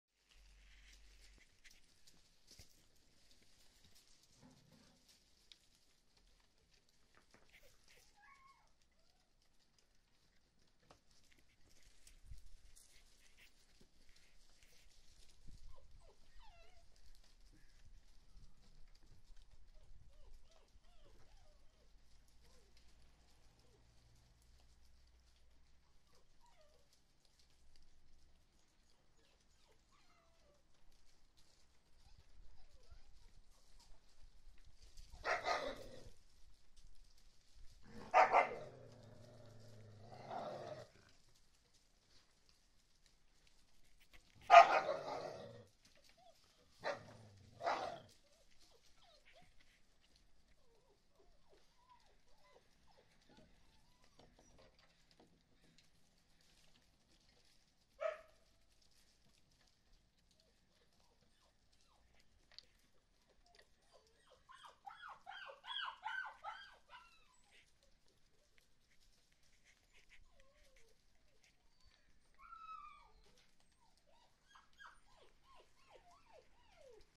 The pups explore the yard in the morning
The pups play under the overhang of the roof to say out of the rain.
Kwimu is always nearby. 8 weeks old.